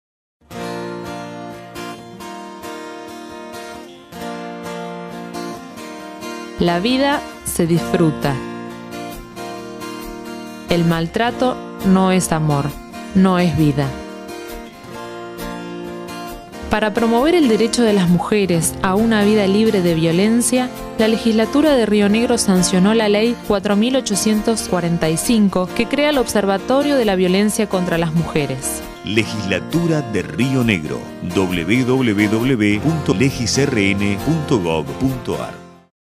Spot Violencia De Género
Para promover el derecho de las mujeres a una vida libre de violencia, La Legislatura de Río Negro sancionó la Ley 4.845 que crea el OBSERVATORIO de la violencia contra las mujeres.